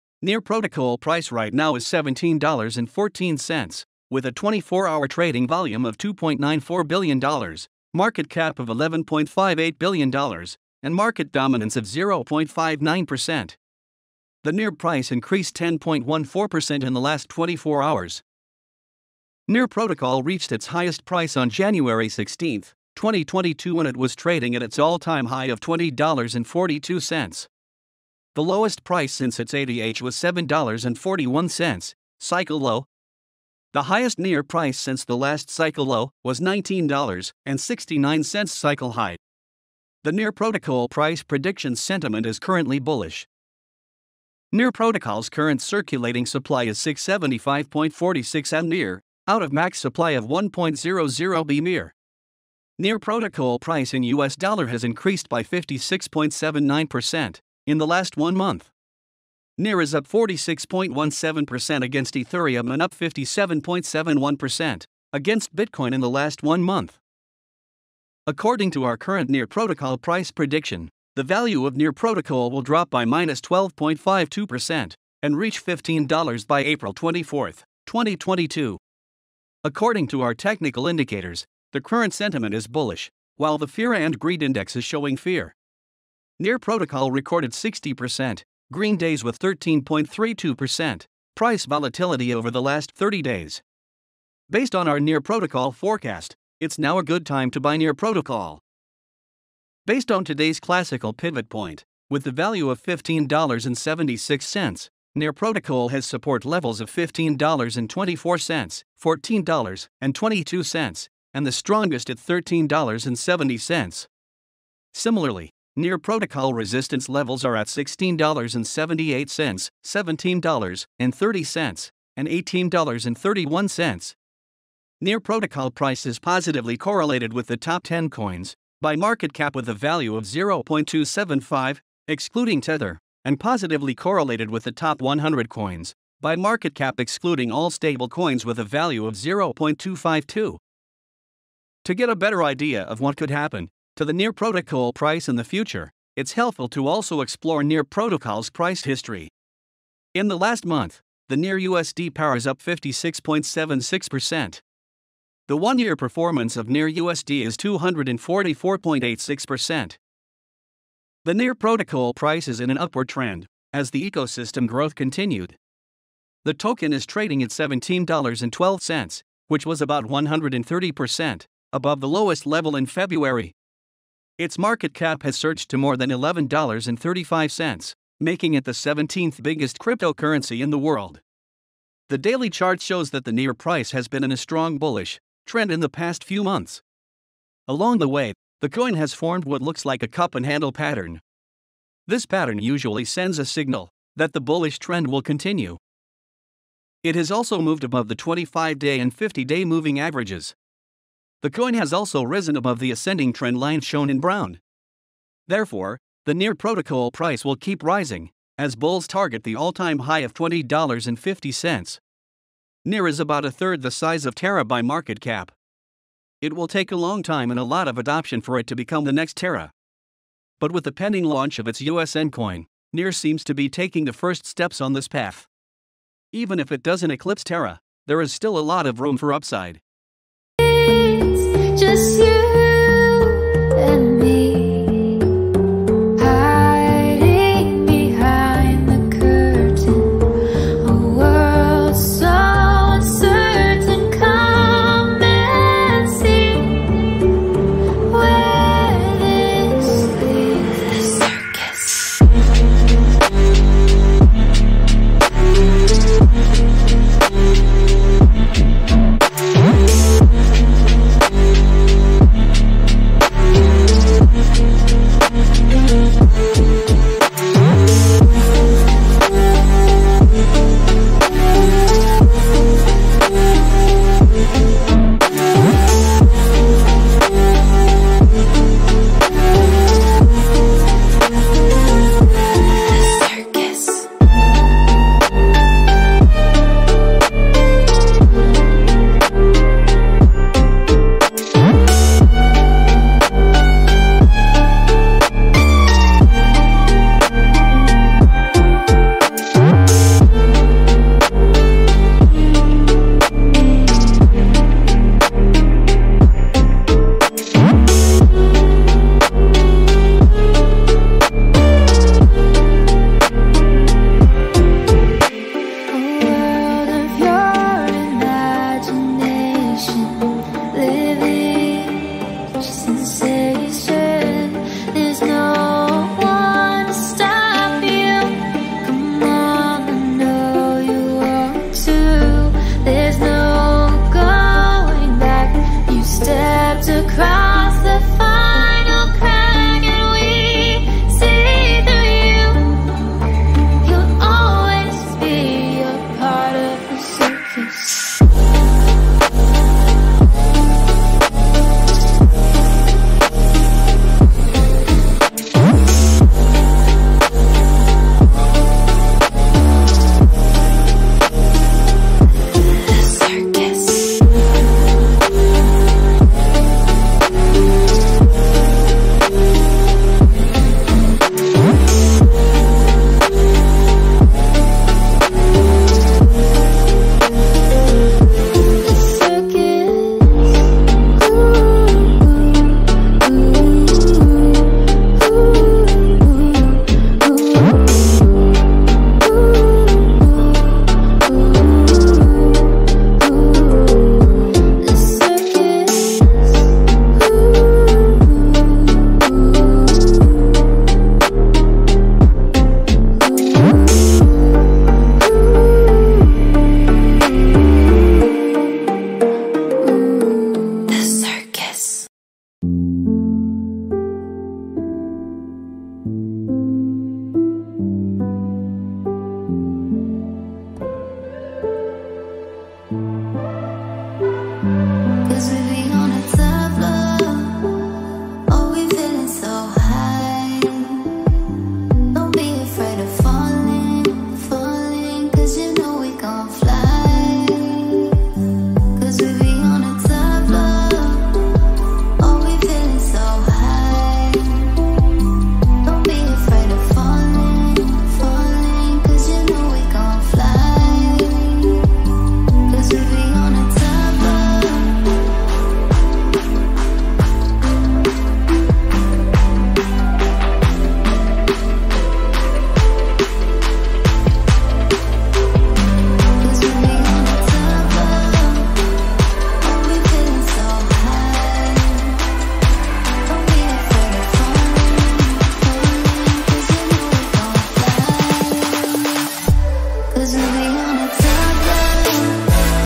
NEAR PROTOCOL MASSIVE PUMP !! NEAR PROTOCOL PRICE PREDICTION
Synopsis: Near Protocol price right now is $ 17.14, with a 24-hour trading volume of $ 2.94B, market cap of $ 11.58B, and market dominance of 0.59%. The NEAR price increased 10.14% in the last 24 hours.

Near Protocol reached its highest price on Jan 16, 2022 when it was trading at its all-time high of $ 20.42. The lowest price since it's ATH was $ 7.41, cycle low. The highest NEAR price since the last cycle low, was $ 19.69 cycle high. The Near Protocol price prediction sentiment is currently bullish.

Near Protocol's current circulating supply is 675.46M NEAR, out of max supply of 1.00B NEAR. Near Protocol price in US Dollar has increased by 56.79%, in the last 1 month. NEAR is up 46.17% against Ethereum and up 57.71%, against Bitcoin in the last 1 month.

According to our current Near Protocol price prediction, the value of Near Protocol will drop by -12.52%, and reach $ 15.00 by April 24, 2022. According to our technical indicators, the current sentiment is Bullish, while the Fear & Greed Index is showing Fear. Near Protocol recorded 60%, green days with 13.32%, price volatility over the last 30 days. Based on our Near Protocol forecast, it's now a good time to buy Near Protocol.

Based on today's classical pivot point, with the value of $ 15.76, Near Protocol has support levels of $ 15.24, $ 14.22, and the strongest at $ 13.70. Similarly, Near Protocol resistance levels are at $ 16.78, $ 17.30, and $ 18.31. Near Protocol price is positively correlated with the top 10 coins, by marketcap with a value of 0.275, excluding Tether, and positively correlated with the top 100 coins, by marketcap excluding all stablecoins with a value of 0.252.

To get a better idea of what could happen, to the Near Protocol price in the future, it’s helpful to also explore Near Protocol’s price history. In the last month, the NEAR USD pair is up 56.76%. The 1-year performance of NEAR USD is 244.86%.

The Near Protocol price is in an upward trend, as the ecosystem growth continued. The token is trading at $17.12, which was about 130%, above the lowest level in February. Its market cap has surged to more than $11.35, making it the 17th biggest cryptocurrency in the world. The daily chart shows that the NEAR price has been in a strong bullish, trend in the past few months. Along the way, the coin has formed what looks like a cup and handle pattern. This pattern usually sends a signal, that the bullish trend will continue.

It has also moved above the 25-day and 50-day moving averages. The coin has also risen above the ascending trendline shown in brown. Therefore, the Near Protocol price will keep rising, as bulls target the all-time high of $20.50. Near is about a third the size of Terra by market cap. It will take a long time and a lot of adoption for it to become the next Terra. But with the pending launch of its USN coin, Near seems to be taking the first steps on this path. Even if it doesn't eclipse Terra, there is still a lot of room for upside.